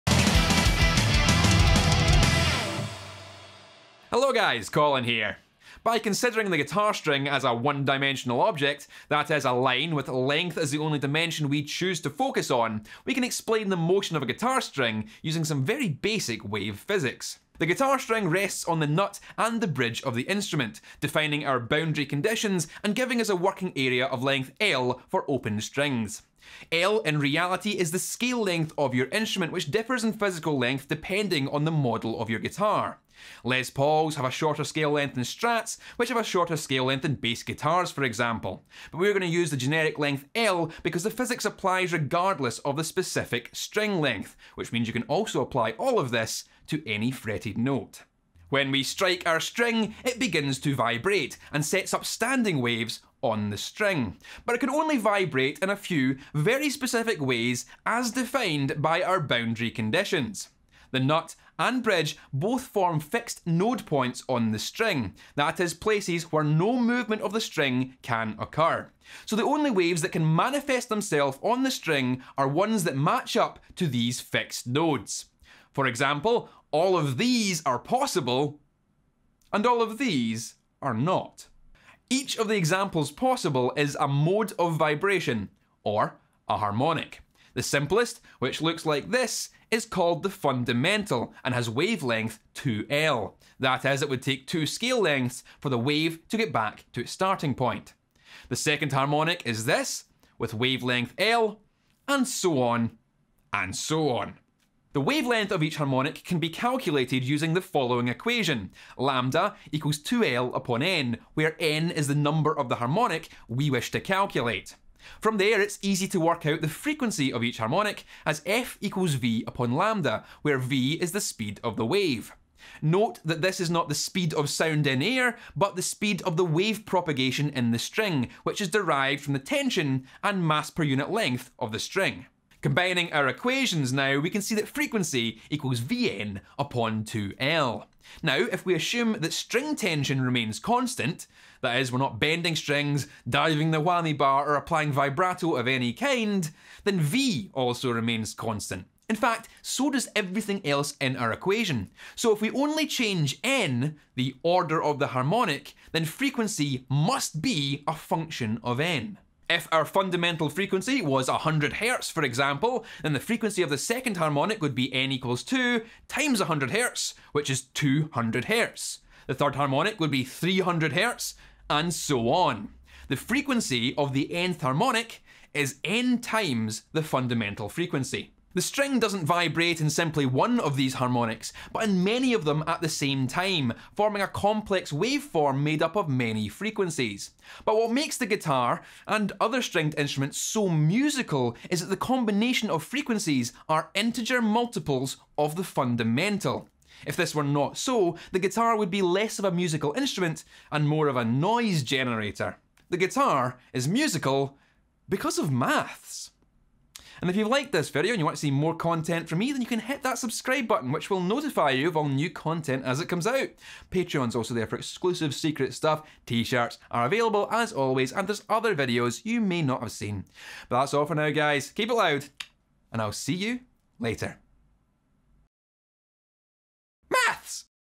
Physics of a Guitar String | Science Minisode
The motion and sound of a guitar string can be explained with some basic wave physics. It won't take long, so let's see how much we can cram into one Science Minisode.

Target Mids available now:
csguitars.co.uk/target-mids

CSGuitars uses:
Dragon's Heart Guitar Picks: